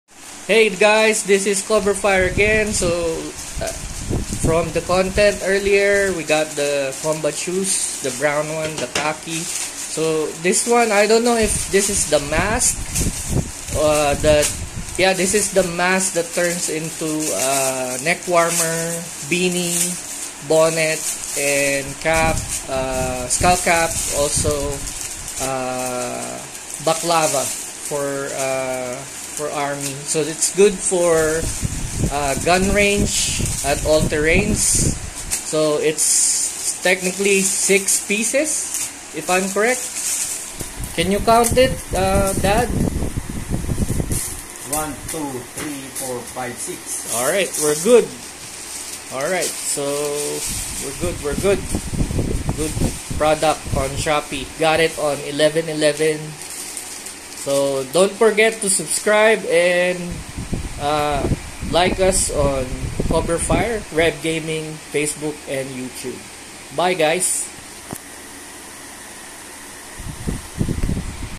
6 form Elastic Camo all design headwear. Only for 106ph
Good for all types of sports, combat sports, and Gun Shooting Range. From Neck Warmer, Face Masks, Beanie, Skullcap, Baclava(Arab Style), Shariane, Foulard, Headband, & Pirate Bandana. likesubscribe REBGaming FB & YT.